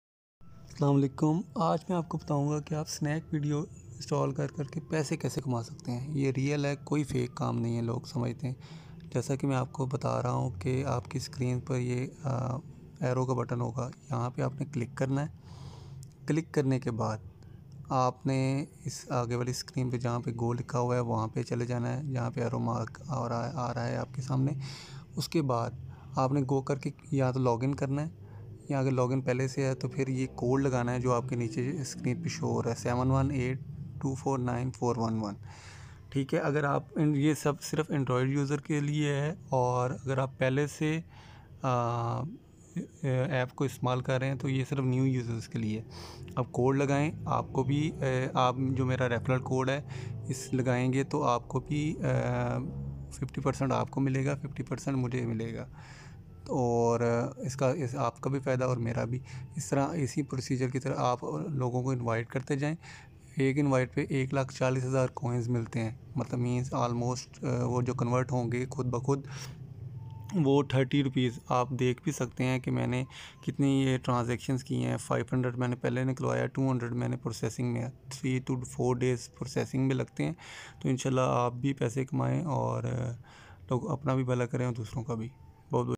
HOW TO Earn from Snack Video App | Earn Real money | HOW TO MAKE ACCOUNT ON SNACK VIDEO APP
Real app real money. Install snack video app. This app will give u real money in 4 days in your jazz cash and easy paisa account so dont b late to install this app.